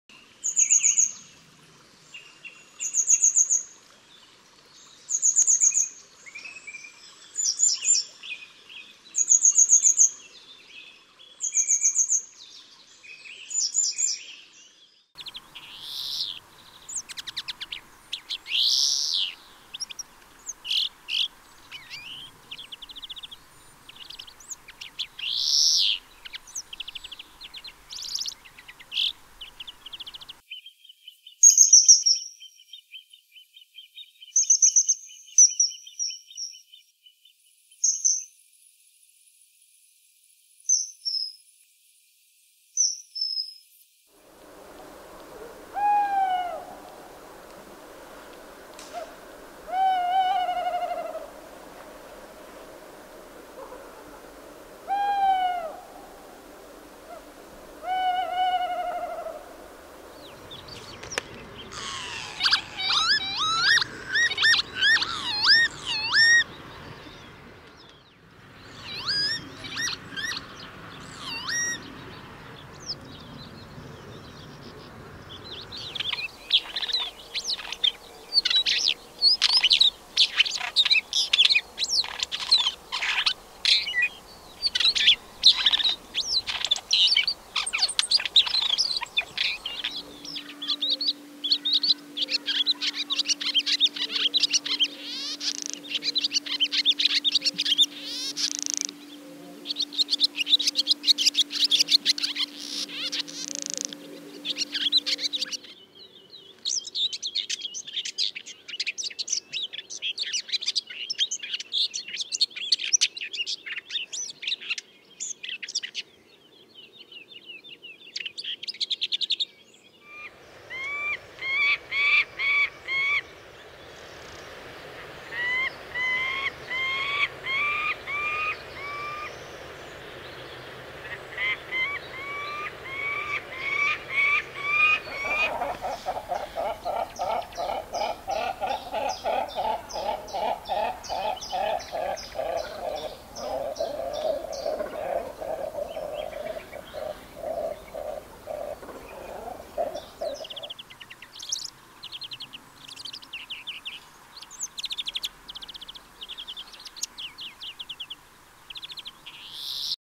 Can you recognize all 10 birds just by sound?
Can you recognize all 10 birds in this video just by sound?

Let me know how many birds you guessed correctly in the comment section!